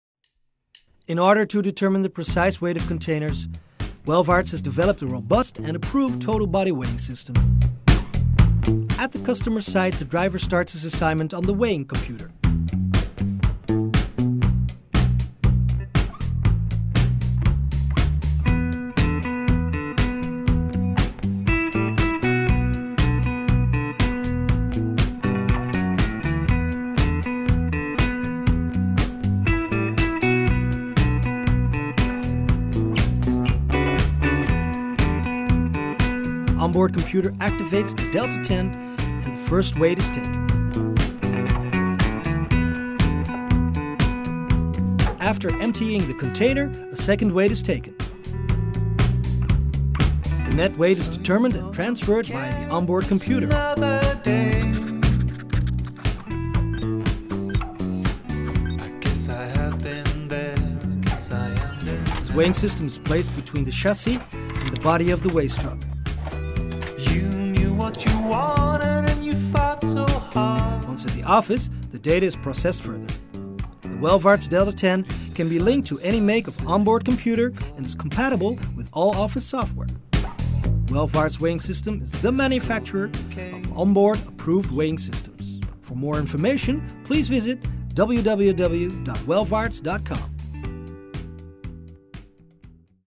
Total body weighing system for garbage truck
Total body weighing system used by garbage trucks.With the total body weighing system the entire construction on the truck is weighed.The waste from the container so well as hand loaded waste can be weighed.